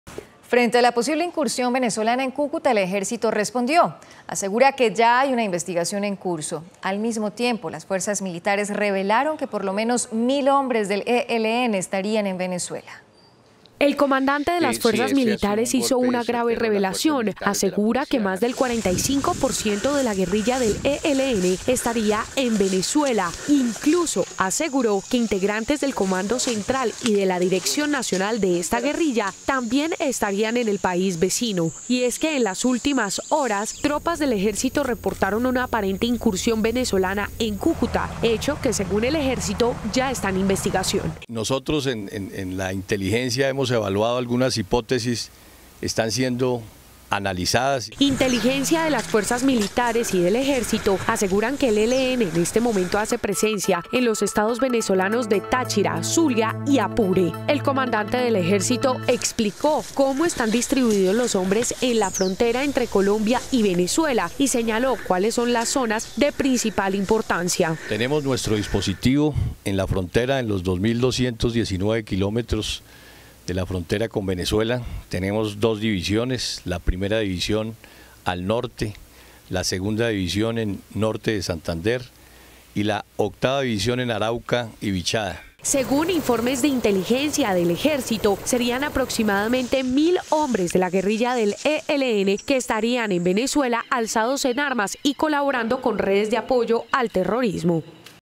45 % del ELN estaría en territorio venezolano
El comandante de las Fuerzas Militares hizo una grave revelación, asegura que más del 45 % de la guerrilla del ELN estaría en Venezuela, incluso aseguró, que integrantes del Comando Central y de la Dirección Nacional de esta guerrilla, también estarían en el país vecino.